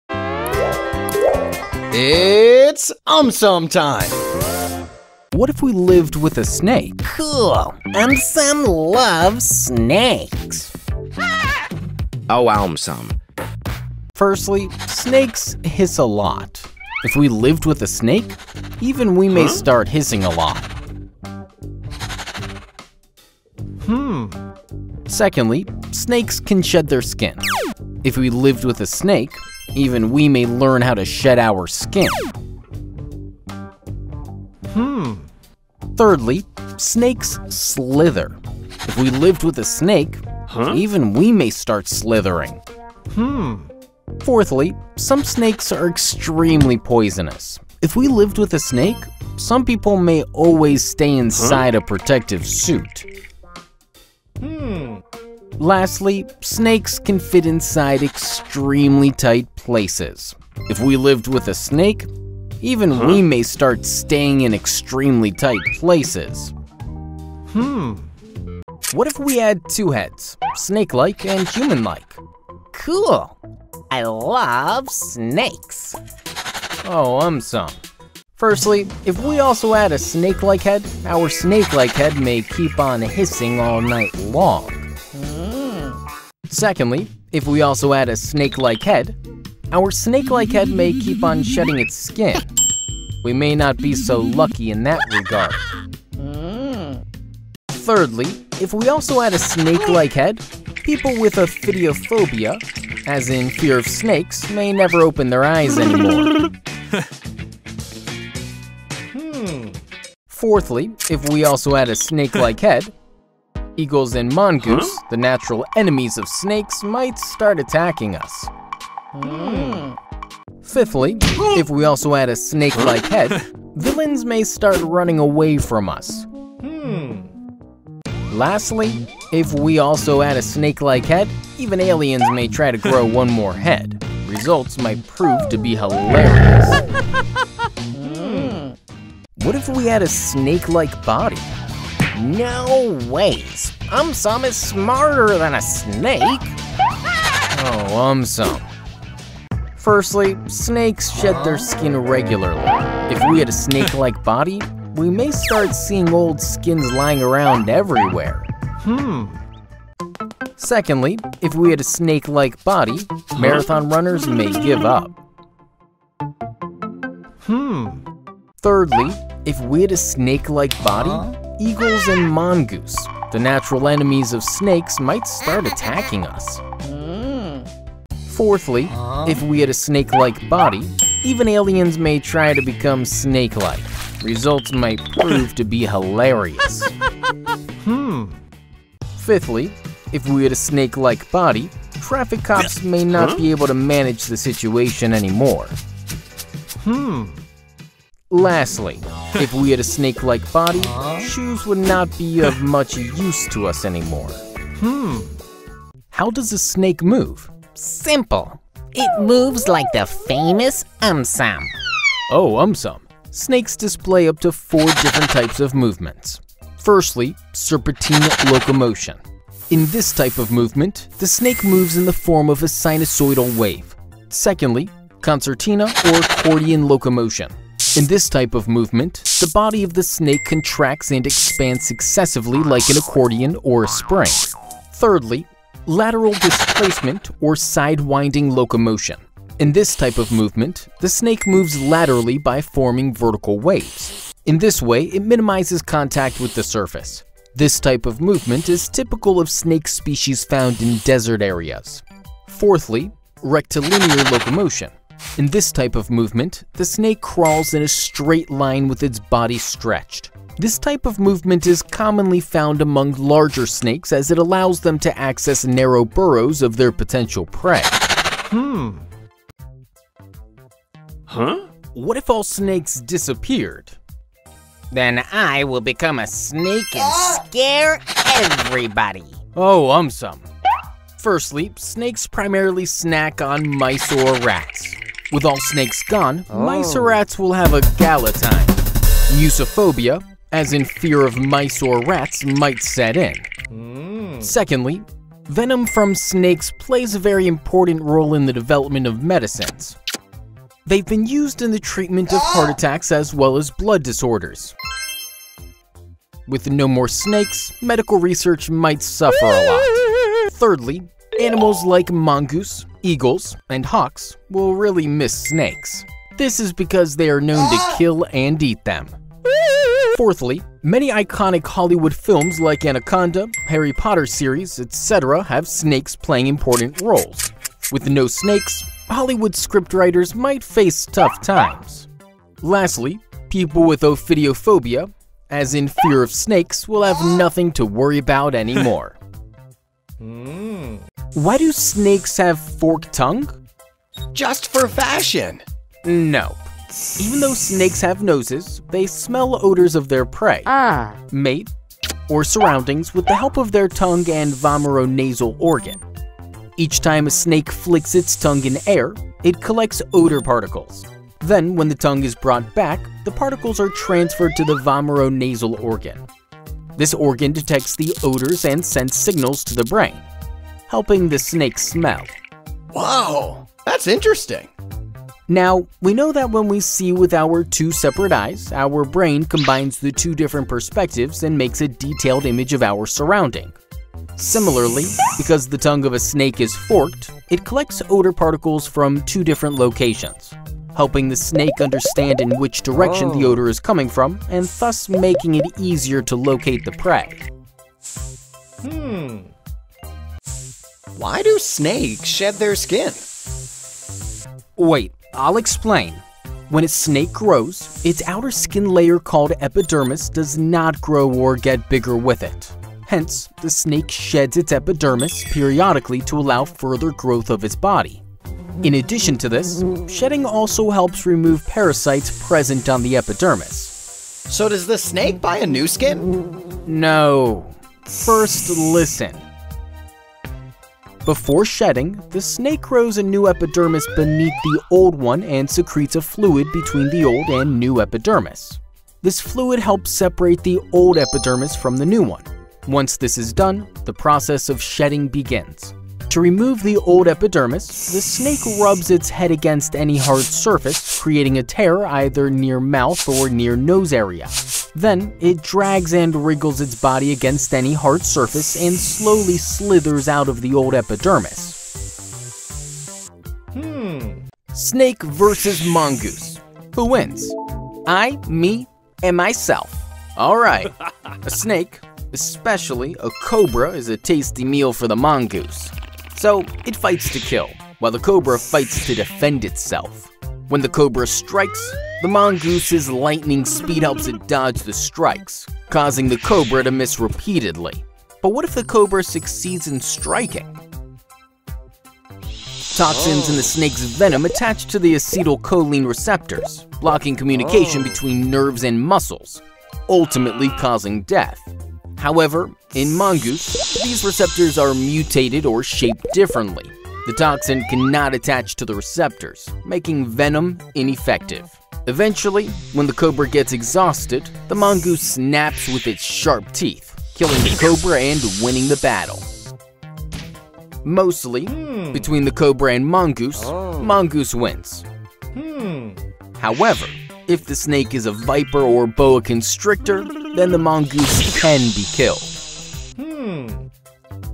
What if we Lived with a Snake? + more videos
What if we Lived with a Snake?
Firstly, Snakes hiss a lot, if we Lived with a Snake, even we may start hissing a lot!
Secondly, Snakes can shade their skin, if we Lived with a Snake, even we may learn how to shed our skin!
Thirdly, Snakes slither, if we Lived with a Snake, even we may start slithering!
Fourthly, some Snakes are extremely poisonous, if we Lived with a Snake, some people may always stay inside a protective suit!
Lastly, Snakes can fit inside extremely tight places, if we Lived with a Snake, even we may start staying in extremely tight places!